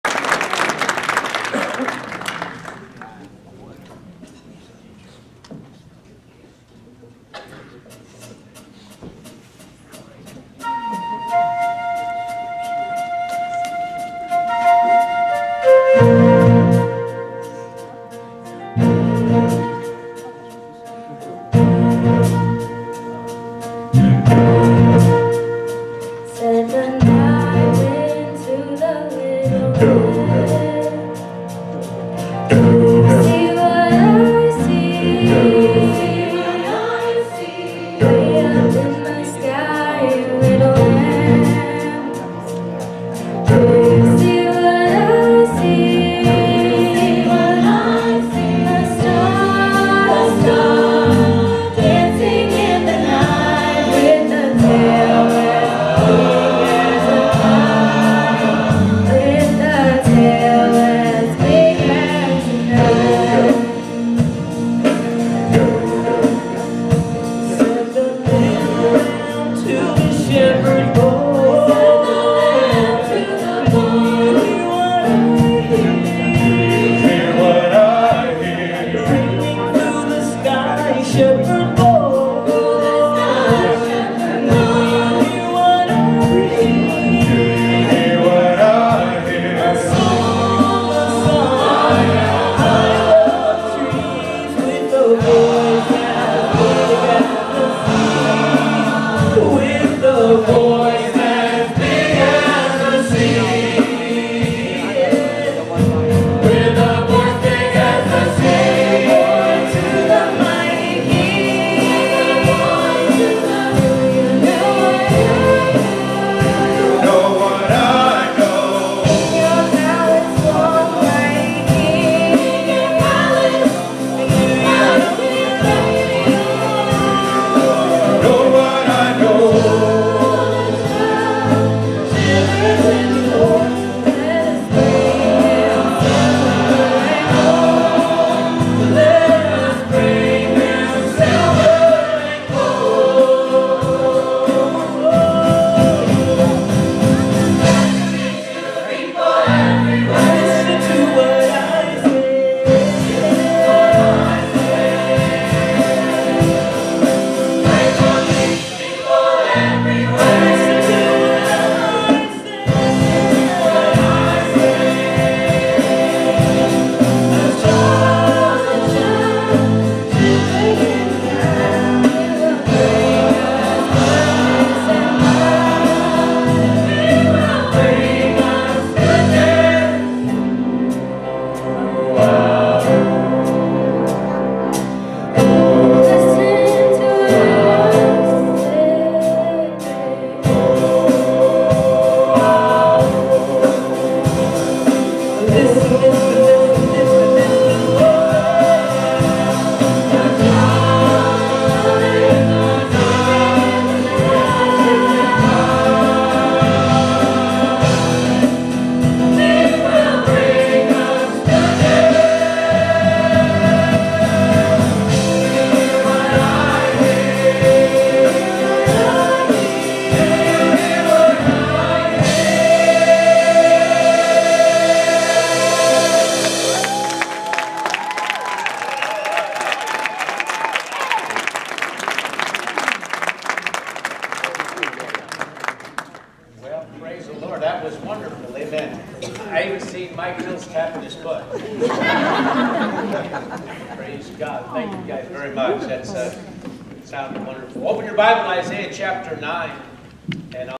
The Door Jacksonville NC Christmas Choir
December 22, 2019 Special Music in Sunday AM service